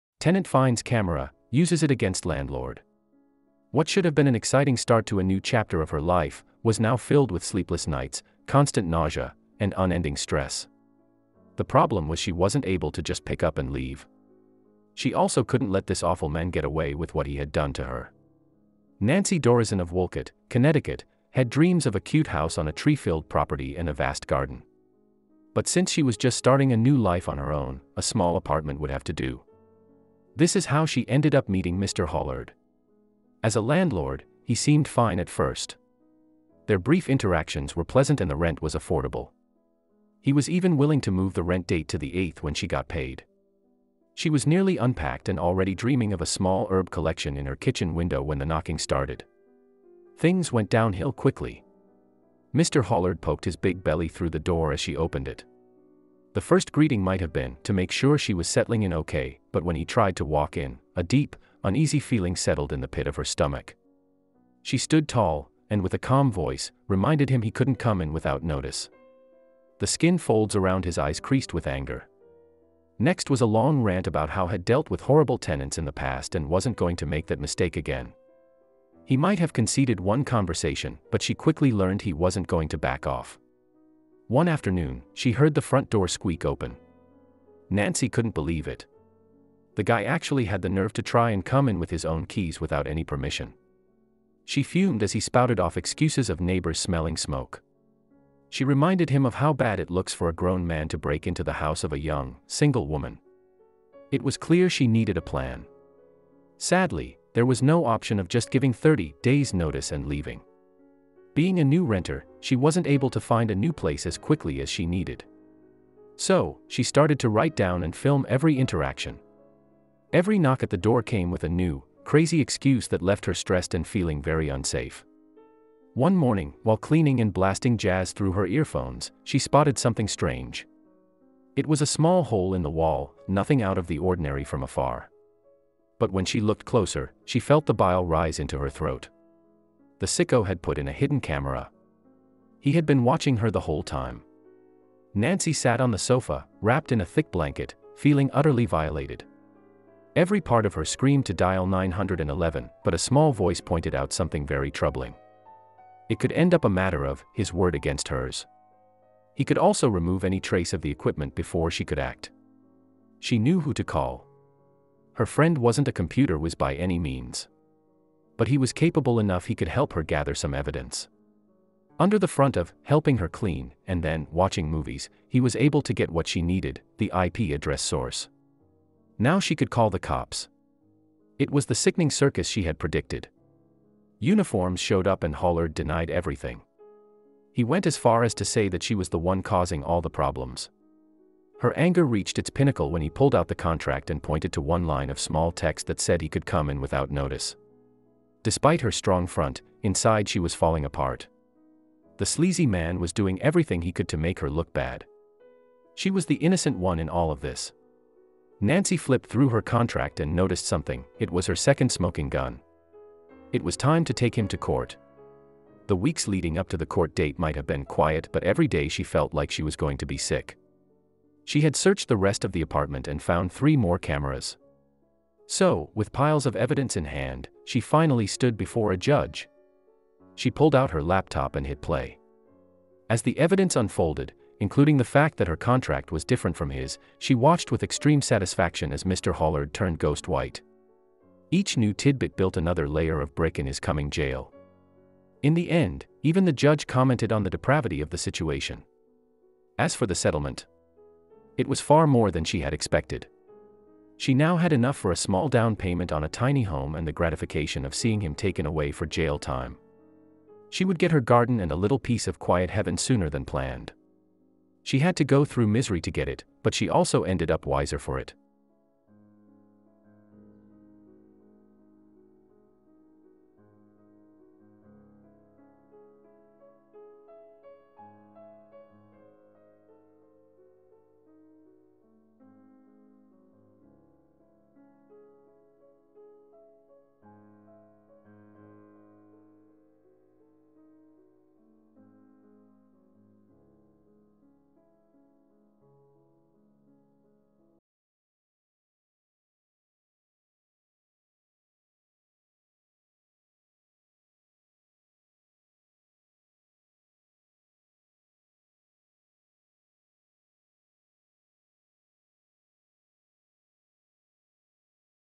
Tenant Finds Camera, Uses It Against Landlord
What should have been an exciting start to a new chapter of her life, was now filled with sleepless nights, constant nausea, and unending stress.
The problem was she wasn’t able to just pick up and leave. She also couldn’t let this awful man get away with what he had done to her.

Nancy Dorison of Wolcott, Connecticut, had dreams of a cute house on a tree-filled property and a vast garden.

But since she was just starting a new life on her own, a small apartment would have to do. This is how she ended up meeting Mr. Hollard. As a landlord, he seemed fine at first.